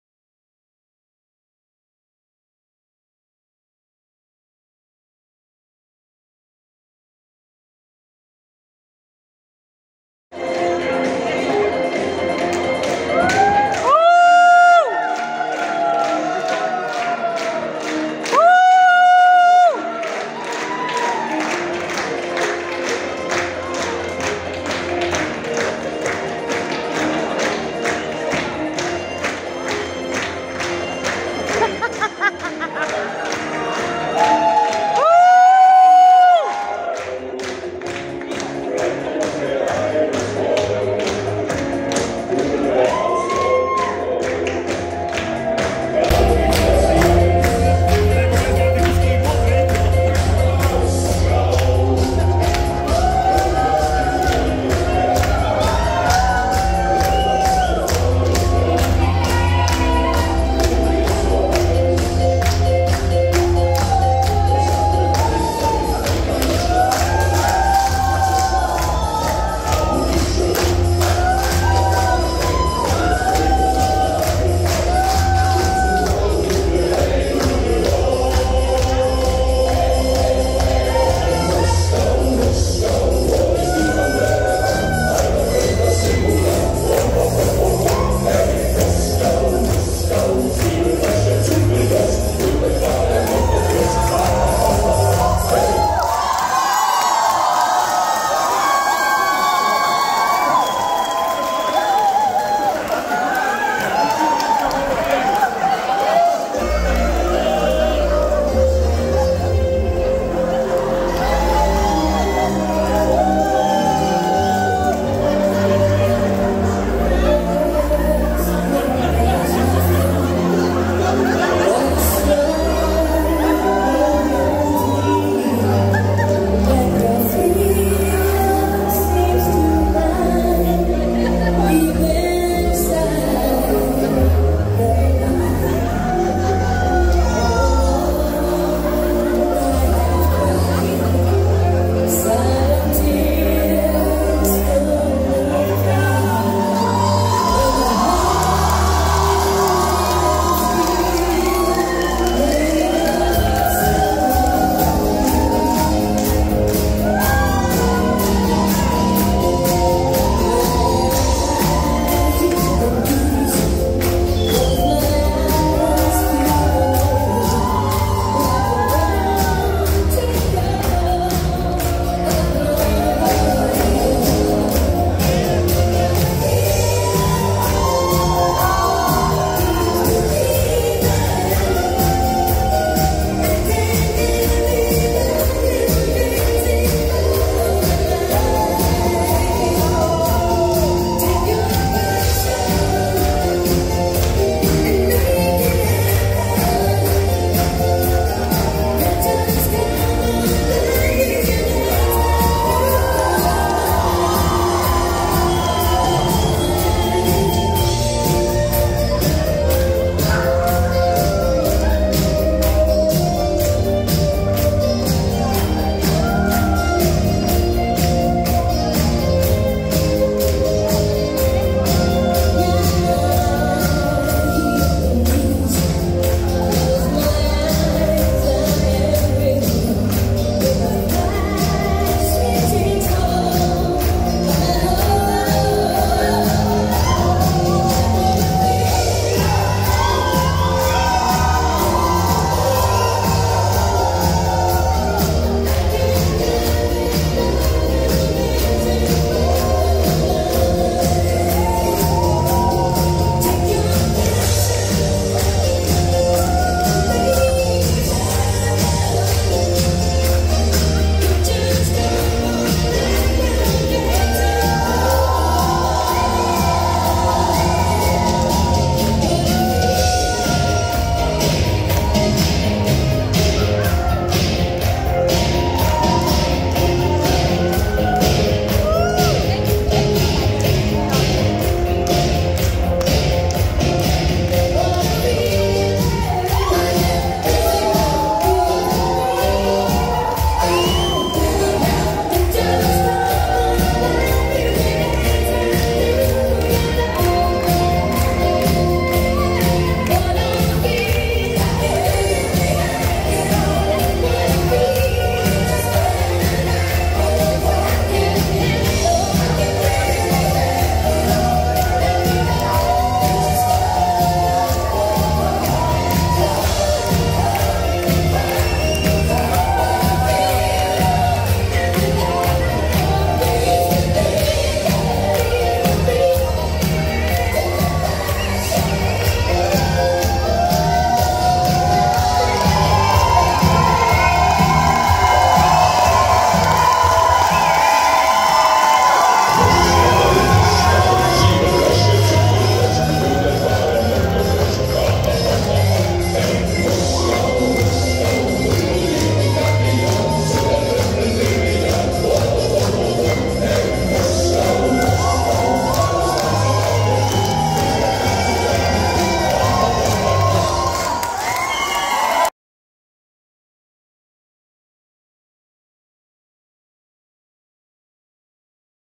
Hasičský ples 2023 - SDH Pavlov
Vystoupení PAVLOVSKÝCH HASIČŮ na hasičském plese 4.2.2023 v Pavlově (okres Žďár nad Sázavou).